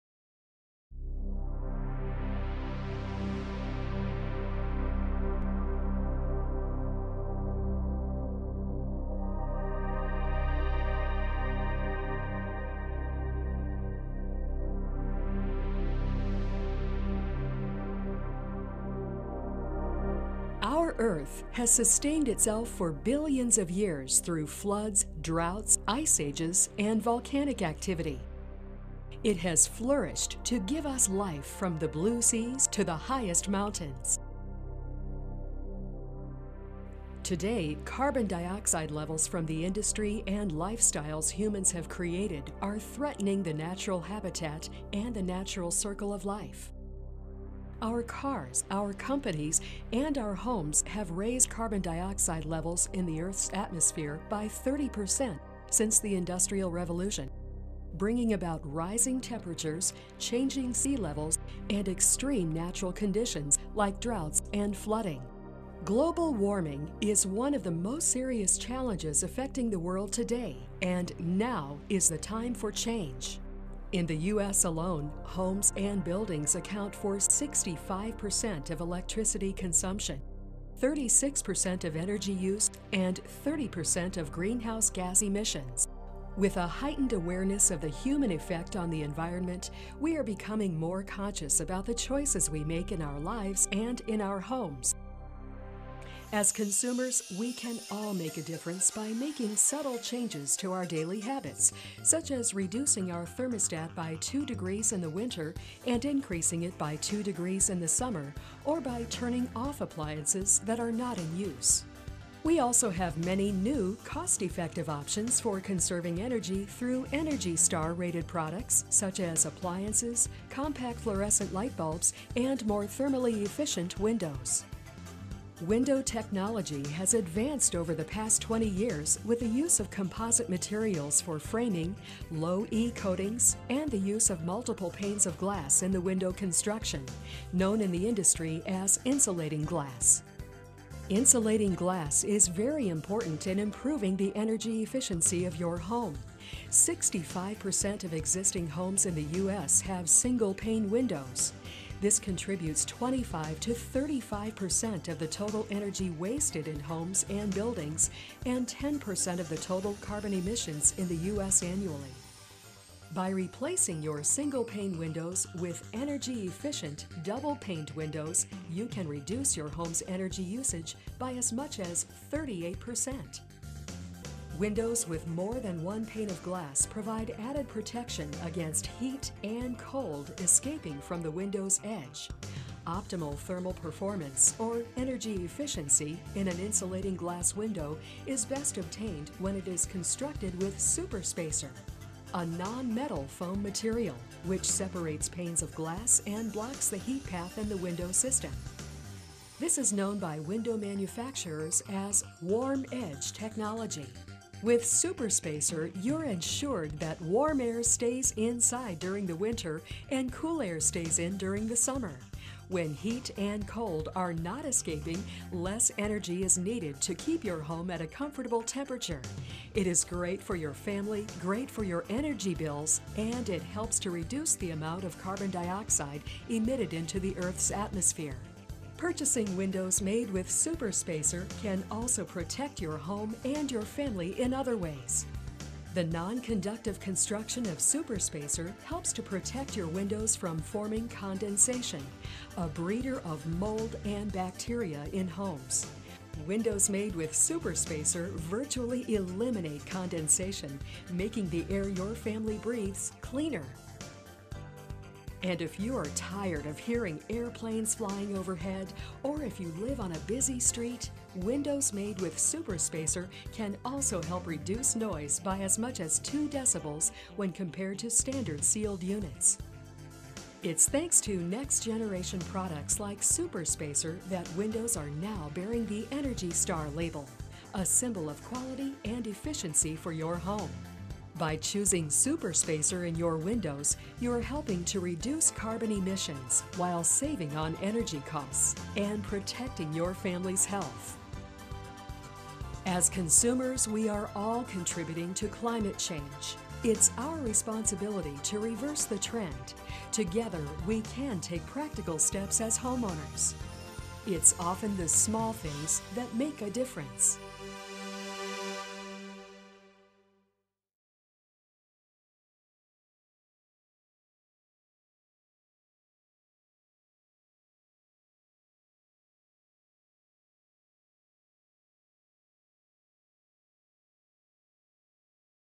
Super Spacer® SustainaView™ Window Technology
With a heightened awareness of the human effect on the environment, we are all becoming more conscious about the choices we make in our lives and in our homes. It's about making changes in even the smallest of ways, including choosing Super Spacer® SustainaView™ Window Technology, which is proven to last longer without needing to be replaced, keeping windows in place longer and out of landfills and promote energy efficiency so that less energy is needed to heat or cool your structure.